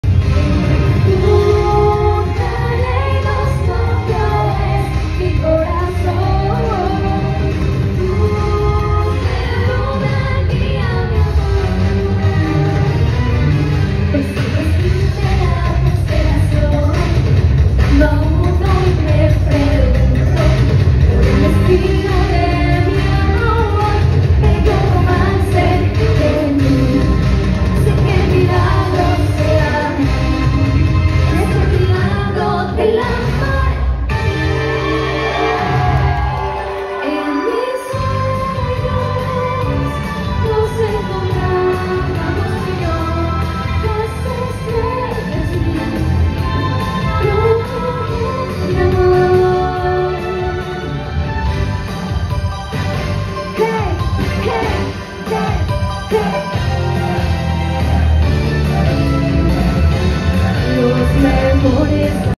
Expo TNT 2022 Annie K. Escenario Cosplay
Gracias TNT y @cosplaytnt por hacerme tan feliz estos 3 días sobre todo hoy gracias por dejarme cantar para este público tan maravilloso y por la oportunidad 🙌🏻💕🥰
Fui tan feliz y soy tan feliz cantando
Una vez más Gracias Gracias Gracias
Dios y el universo son buenos💕